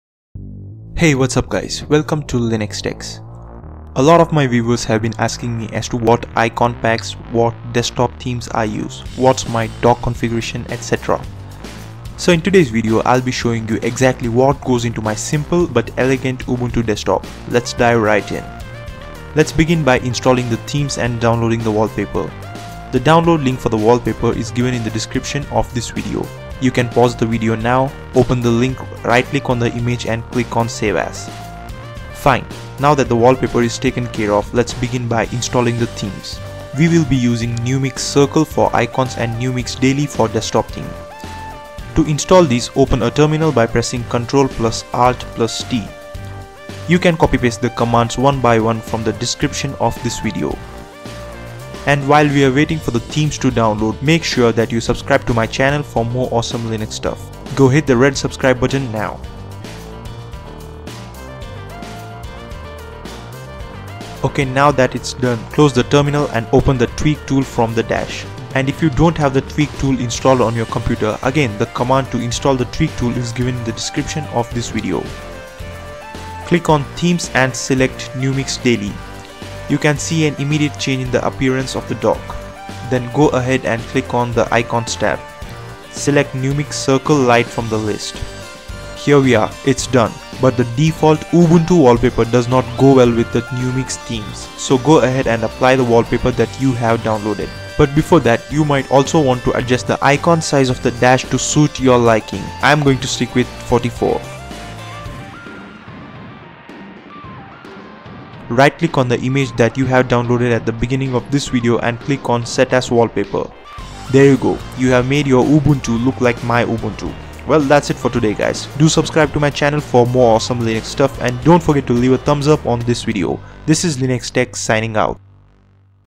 Linux desktop customisation #1- Chill Coastal Ubuntu
Numix Theme installation commands:
sudo add-apt-repository ppa:numix/ppa
sudo apt-get update
sudo apt-get install numix-gtk-theme numix-icon-theme-circle

Unity Tweak Tool installation command:
sudo apt install unity-tweak-tool

A lot of my viewers have been asking me, as to what icon packs, what Desktop Themes I use, what's my dock configuration et cetera.
So in today's video, I'll be showing u exactly what goes into my simple but elegant ubuntu desktop.